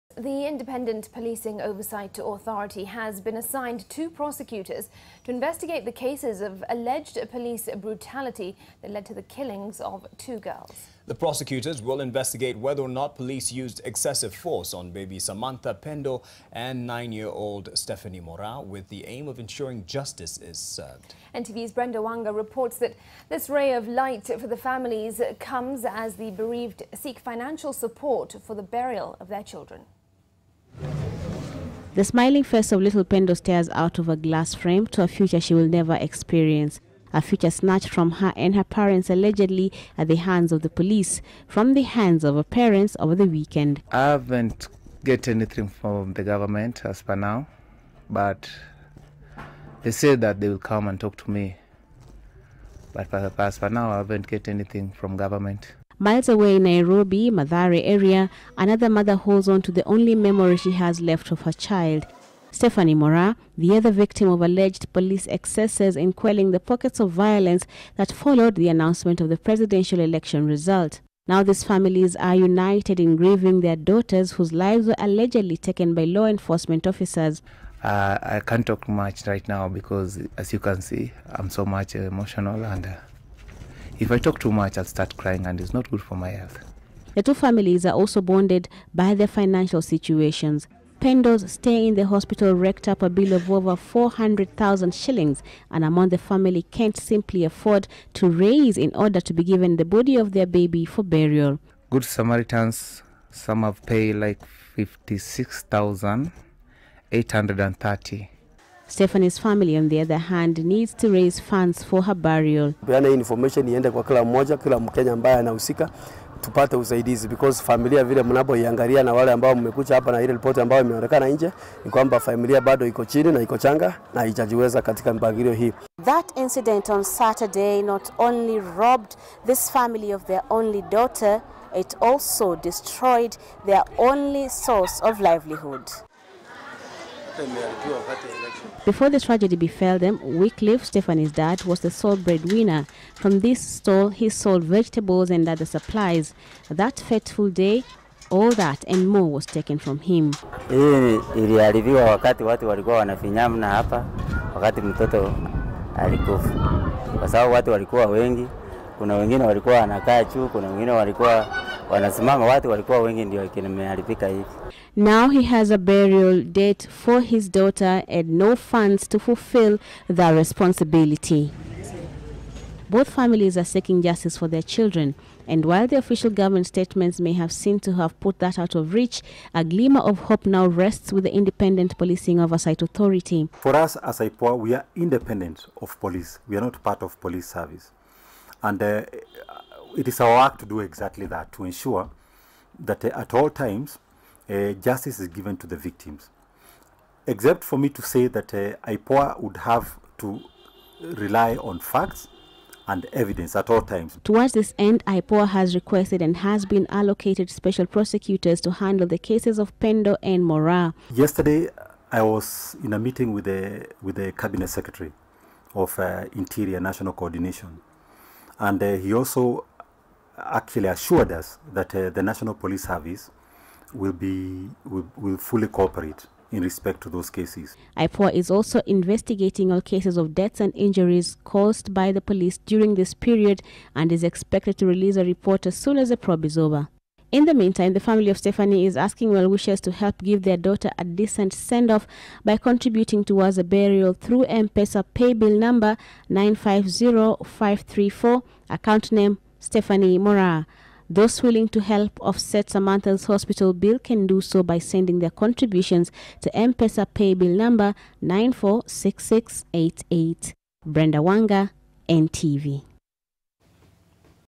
IPOA probing cases of police excesses during protests
The Independent policing oversight authority has been assigned two prosecutors to investigate the cases of alleged police brutality, that led to the killings of two girls. The prosecutors will investigate whether police used excessive force on baby Samantha Pendo, and nine year old Stephanie Moraa, with the aim of ensuring justice is served. NTV ’s Brenda Wanga reports that this ray of light for the families come as the bereaved seek financial support for the burial of their children.

Courtesy of NTV